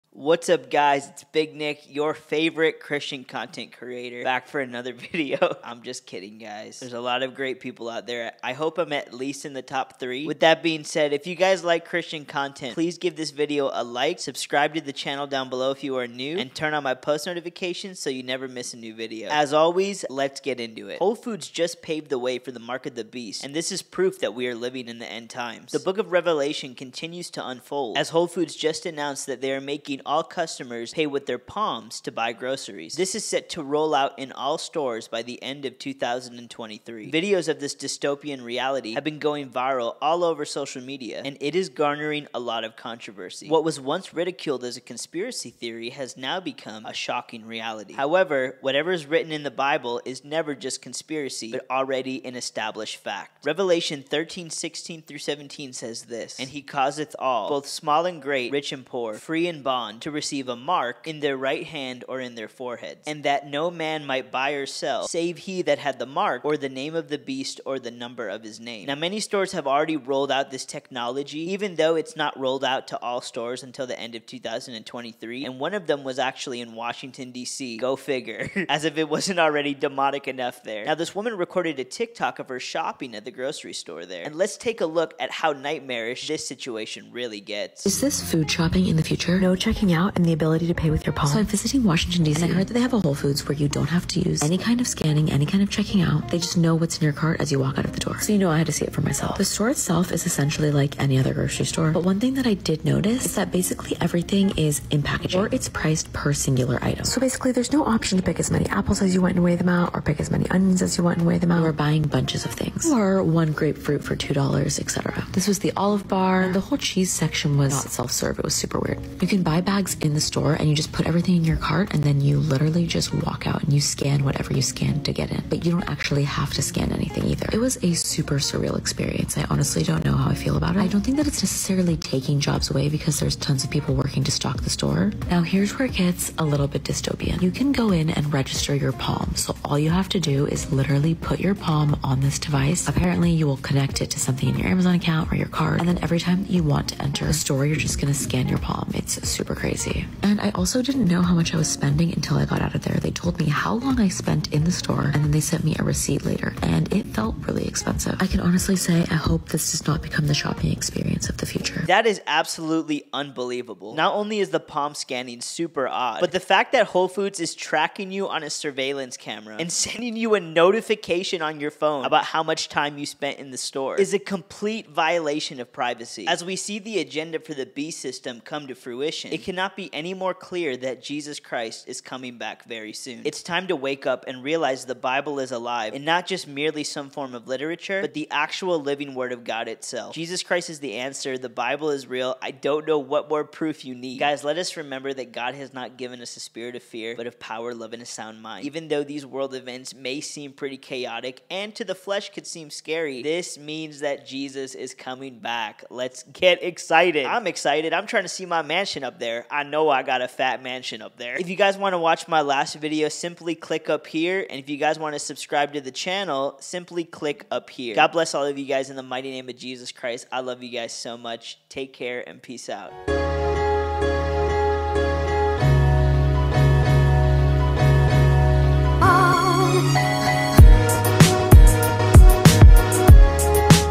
Whole Foods Announces MARK OF THE BEAST Technology! Biblical Prophecy Unfolding...
Whole Foods just announced a shocking a new update for payments in their stores, and it is looking like a precursor for the mark of the beast to be implemented in society. I cover this bizarre new dystopian reality in detail and share a biblical perspective on why this is happening.